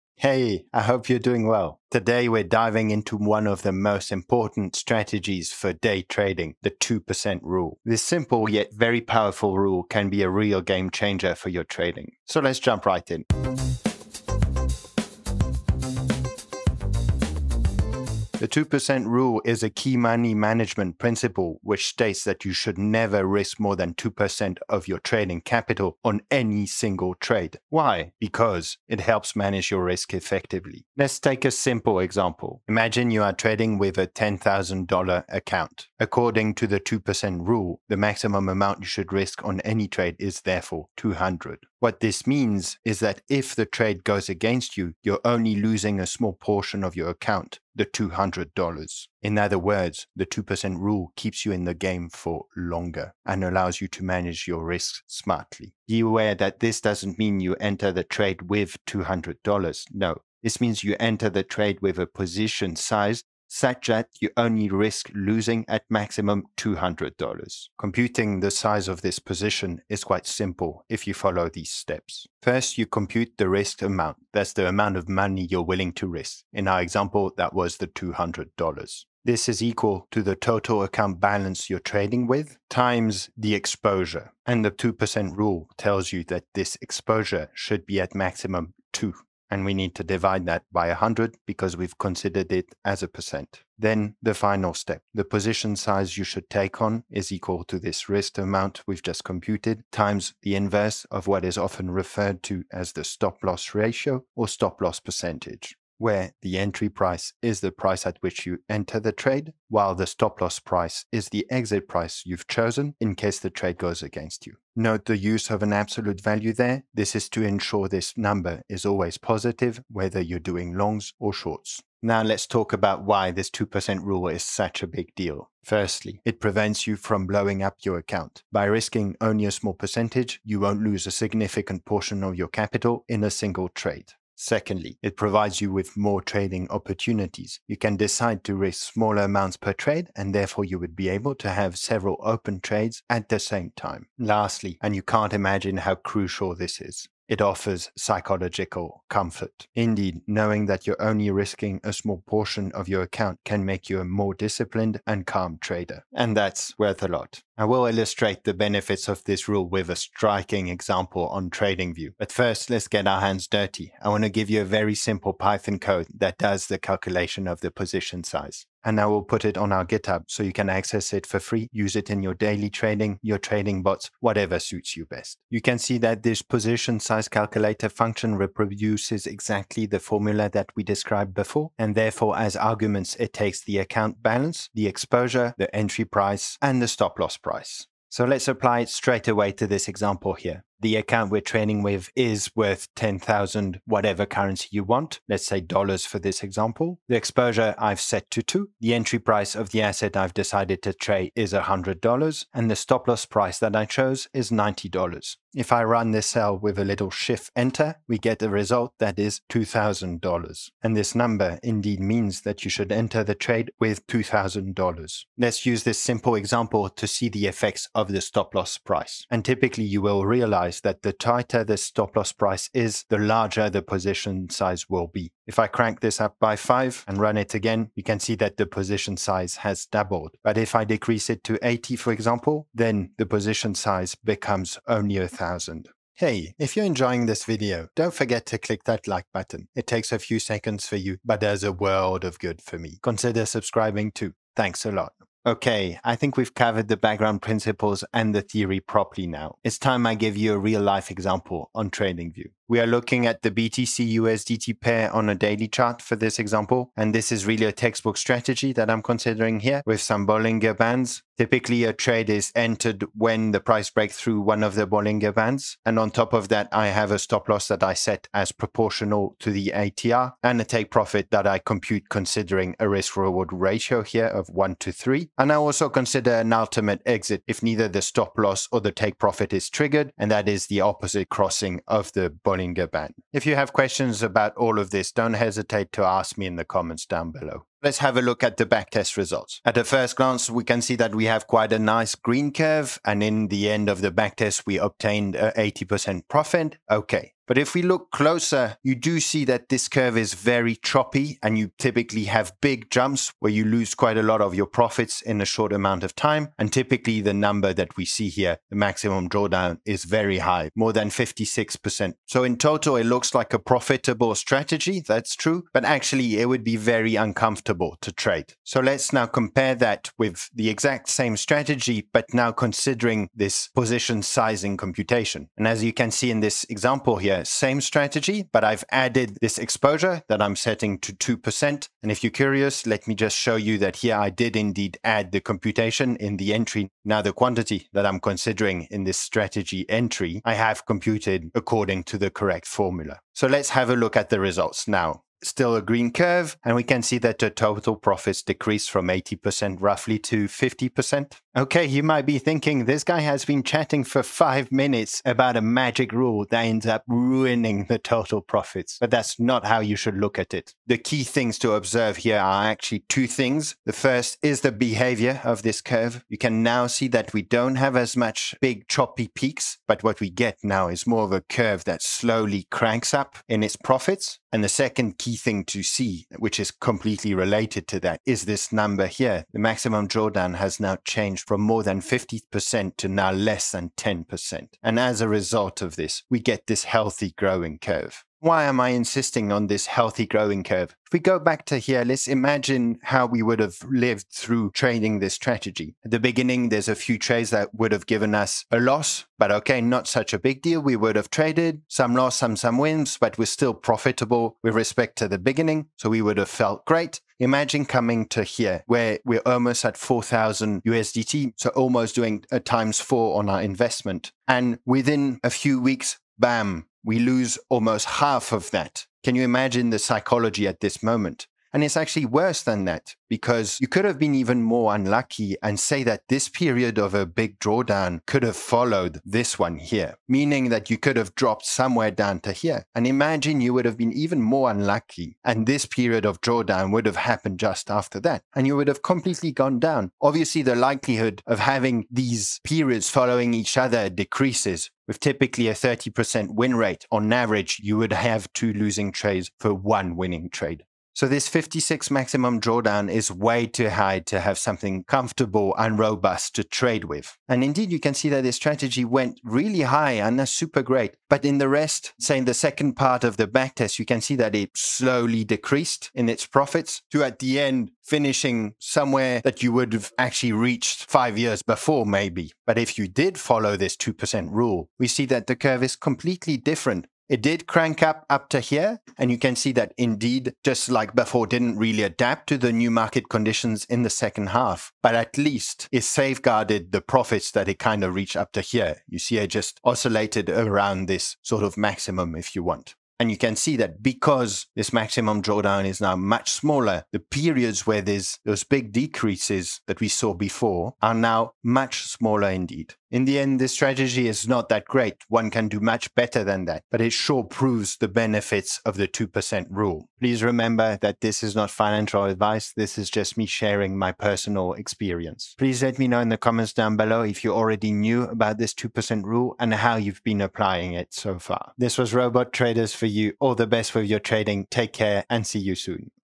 Position Size Calculator And Risk Minimisation
Find out about the power of the 2% rule in trading through this pedagogical video. Here is the art of risk management by limiting your exposure to a small percentage of your capital. Learn how this strategic approach helps protect your account from substantial losses and fosters long-term stability. Join me as I visually demonstrate this essential principle, providing you with valuable insights for making more informed trading decisions.

     Timestamps
00:18 What it is
02:05 Why it is so important
02:48 Simple position size calculator in Python
04:12 TradingView illustration